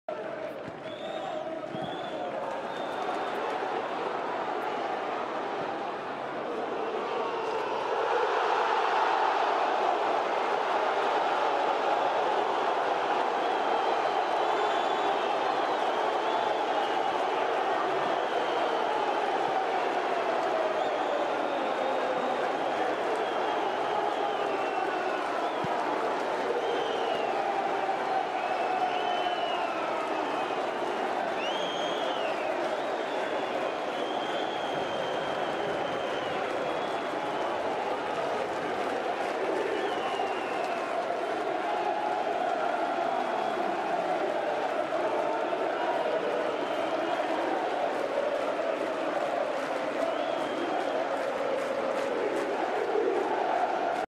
Goal | Golo Osmajic: FC Vizela (3)-0 Vitória SC (Liga 22/23 #14)
FC Vizela: Golo de Osmajic!